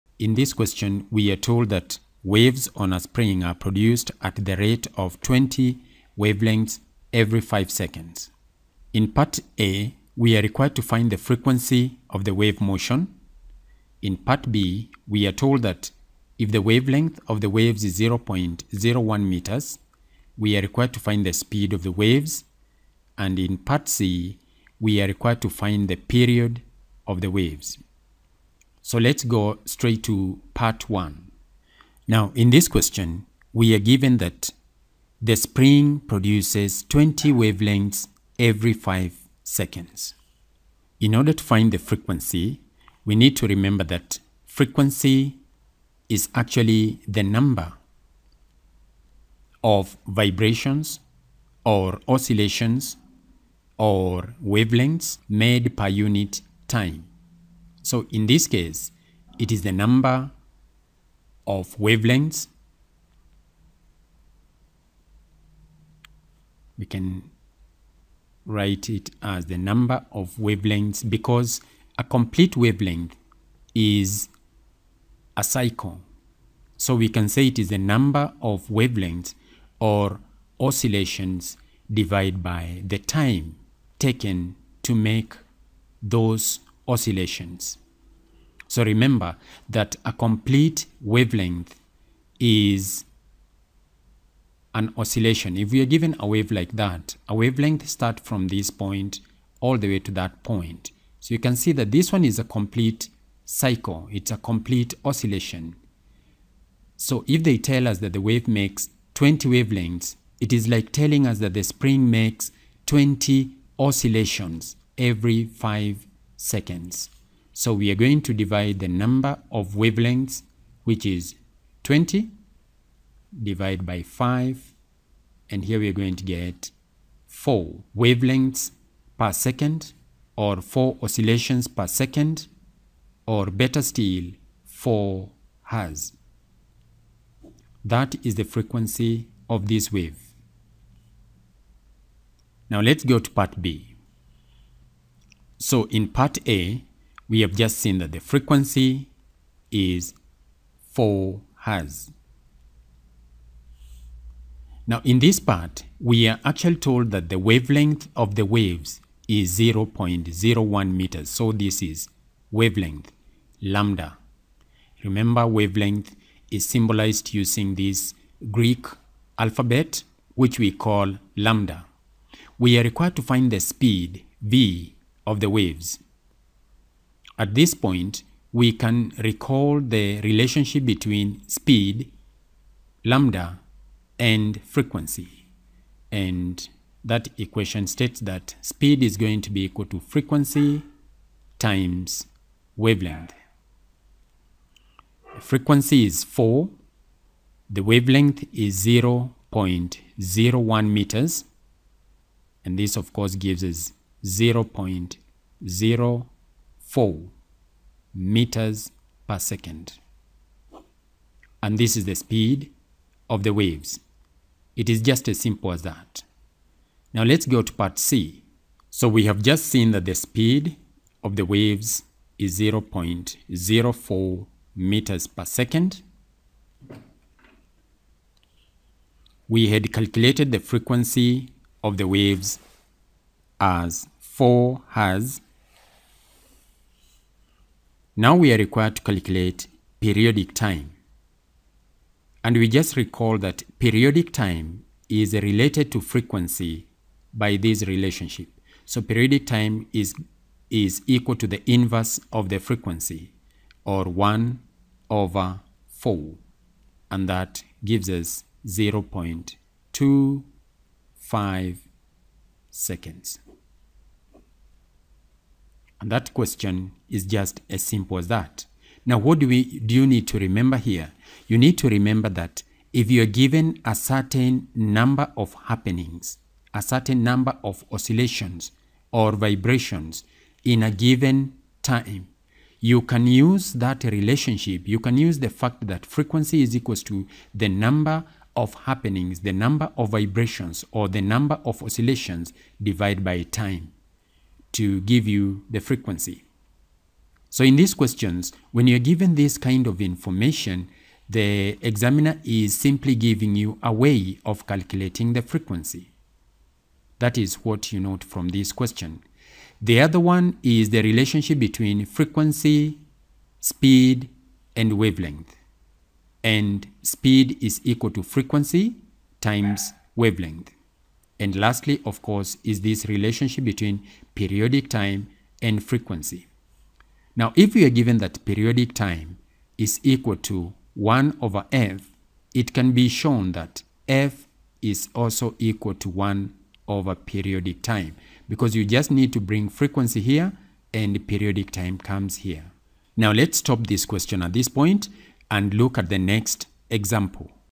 Calculating frequency and speed of a wave
This video is all about calculating frequency, wavelength and period of waves on a spring